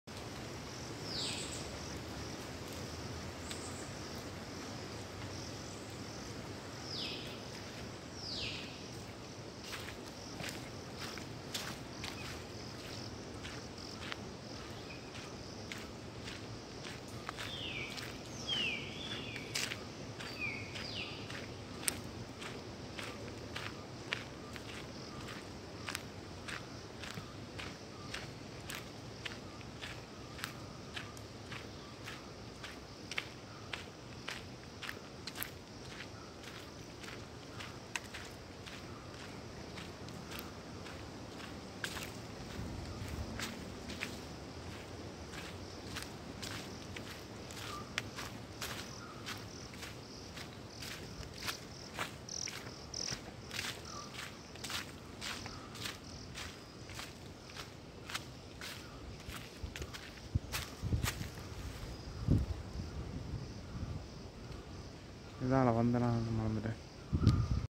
அழகான பயணங்கள் சிறந்த அனுபவத்தை தரும் | Beautiful journeys make for great experiences
அழகான காட்டுக்குள் ஒரு பயணம் . இது இலங்கையில் கண்டி நகரத்தில் உள்ளது.
இது இயற்கையான காடு.

இது காலை 8.00 am to 6.pm வரை சென்று பார்வை இடலாம் . இதற்கு 75.00 ரூபா இலங்கை பணம் செலுத்த வேண்டும் .

udawattakele Kandy national forest

A journey into the beautiful forest.  It is located in the city of Kandy in Sri Lanka.
  It is a natural forest.

 It can be visited from 8.00 am to 6.pm.  75.00 Sri Lankan rupees should be paid for this.